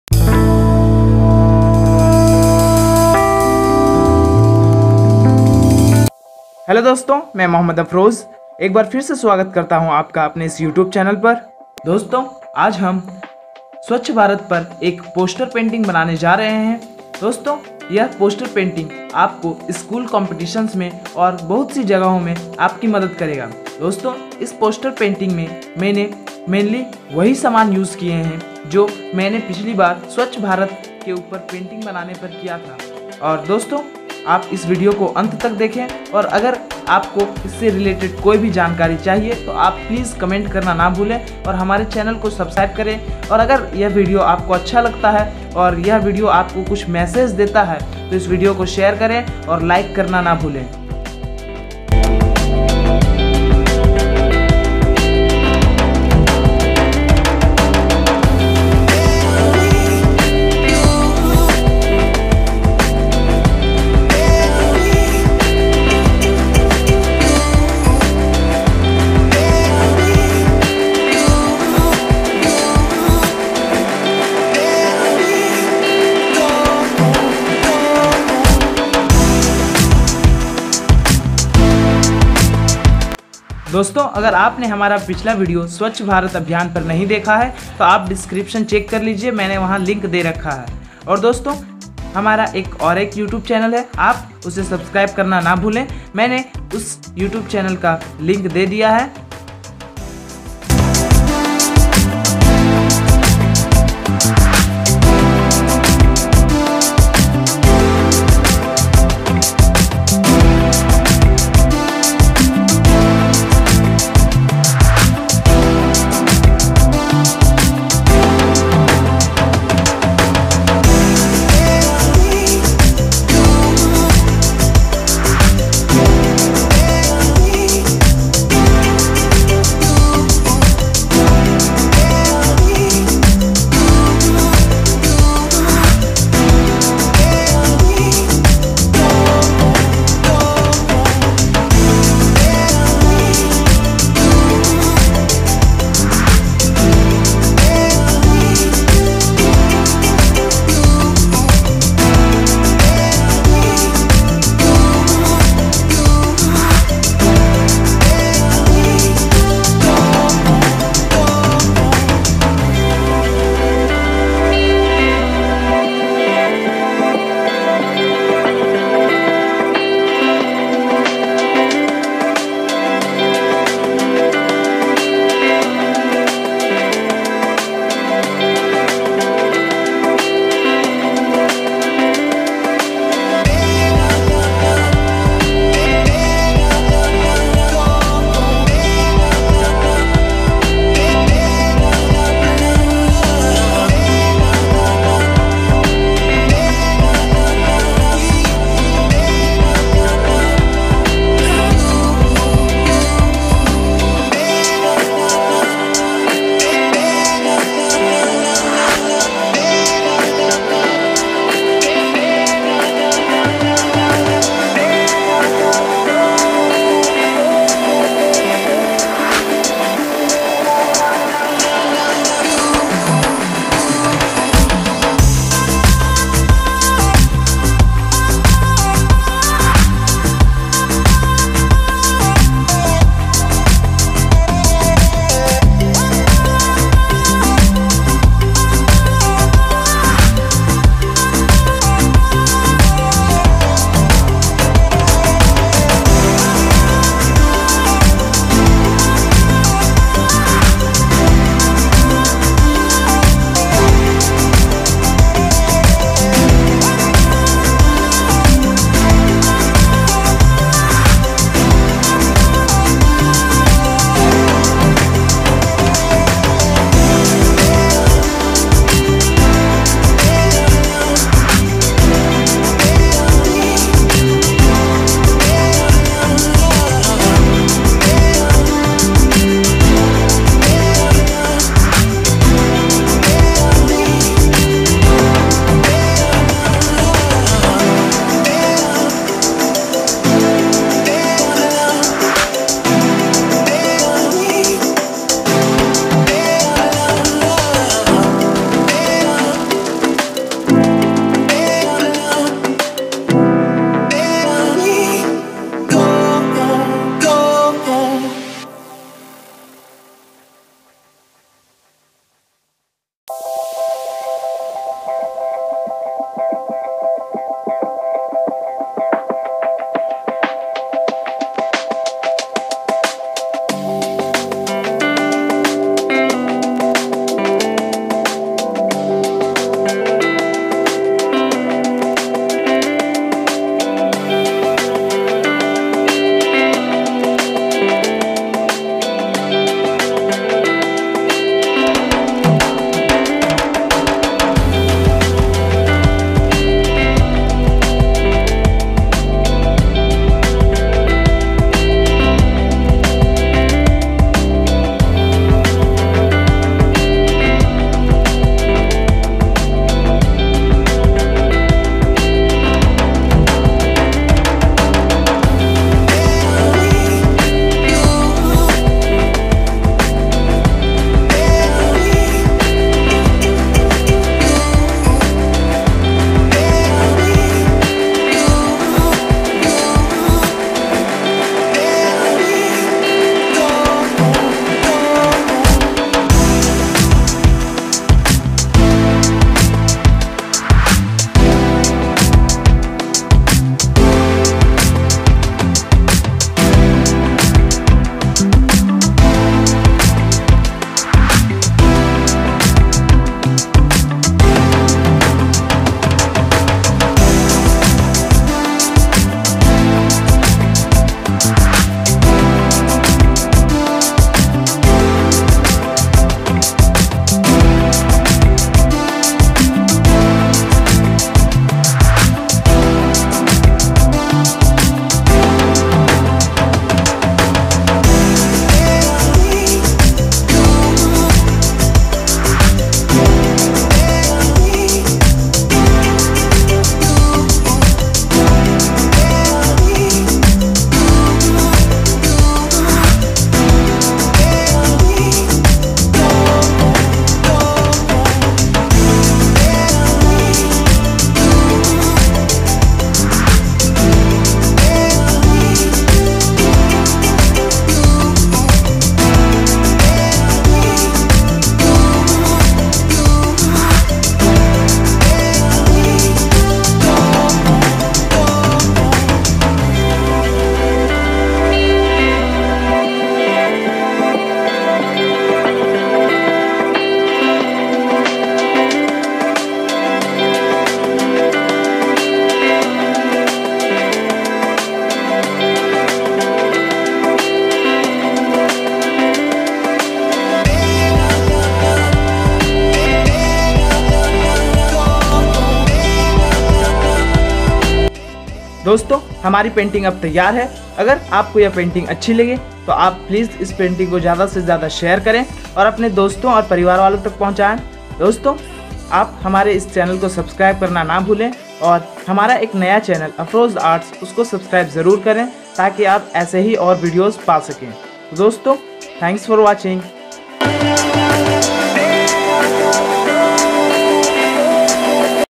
How to make Poster on the topic of "Swachh Bharat Abhiyan" in easy way step by step
hello friend and today I am going to show you how to draw a poster painting on the topic of swachh Bharat Abhiyan a lot of people have commented that please make a video on how to draw poster painting on the swachh Bharat Abhiyan so I have done it so I hope you all guys be like this video and please share this video and if you think that this is providing you some message then please share it with your friend and your family members so that they also can get some message from it.